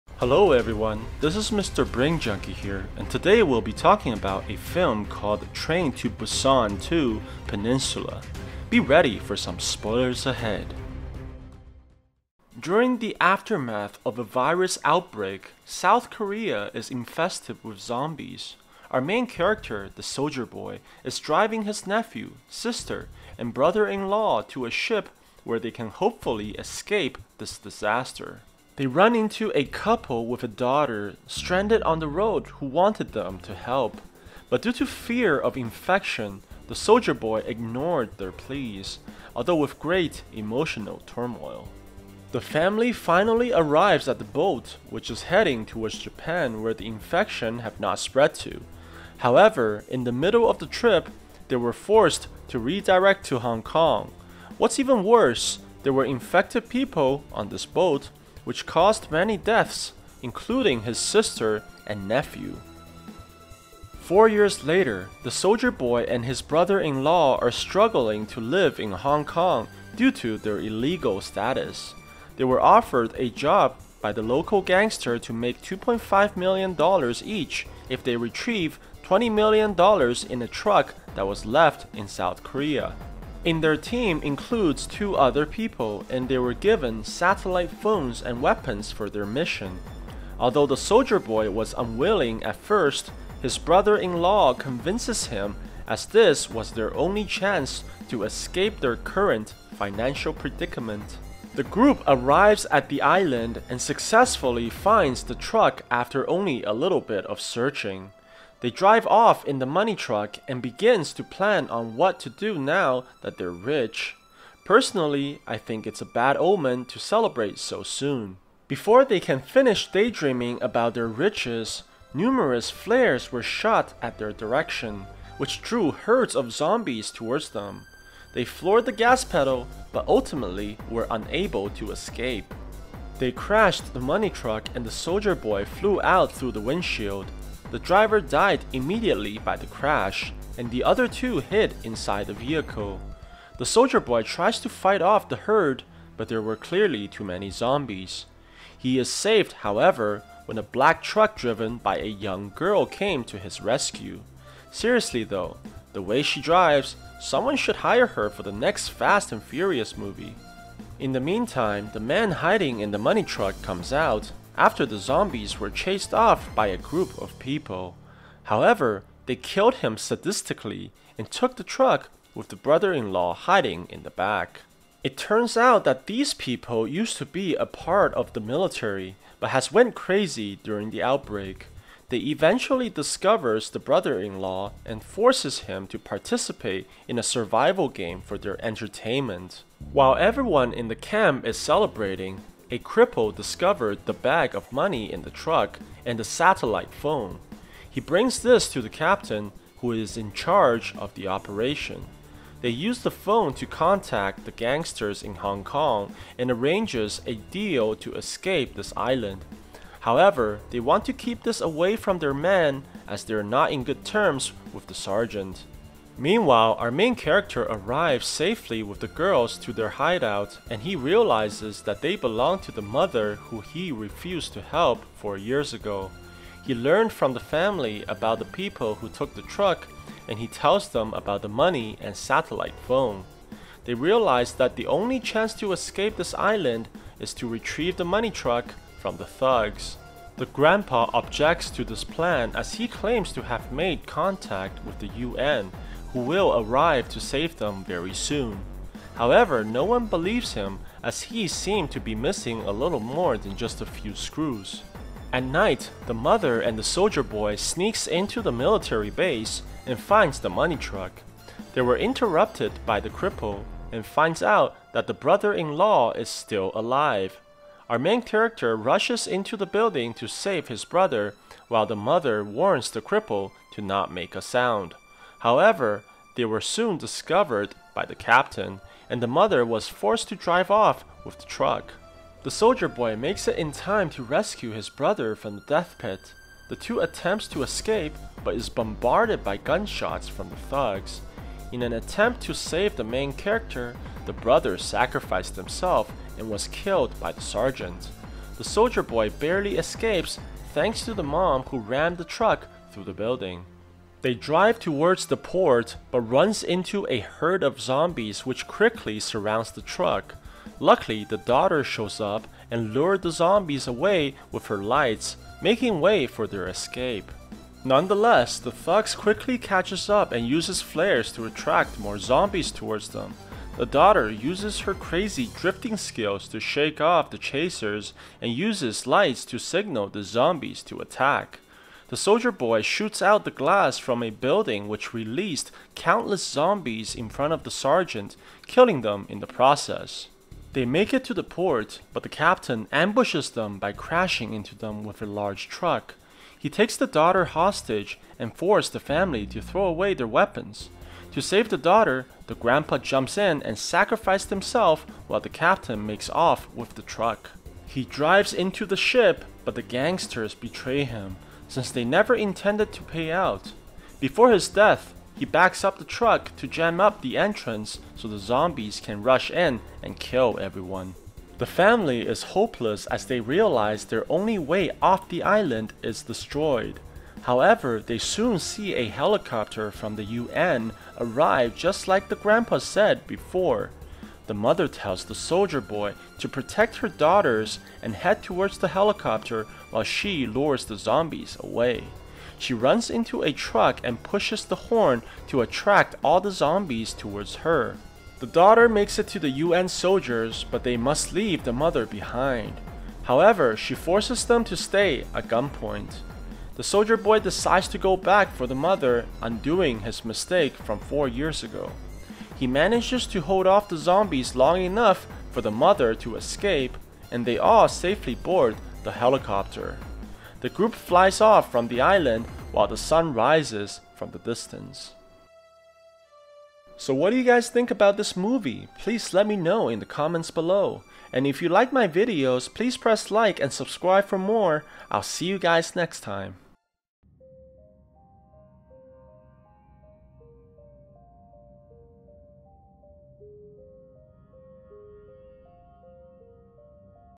Man Fights In Zombified World To Get Rich!
A man fights in a zombified world in order to get rich.  Let me know which movie you want to have it's story recapped in the comments below!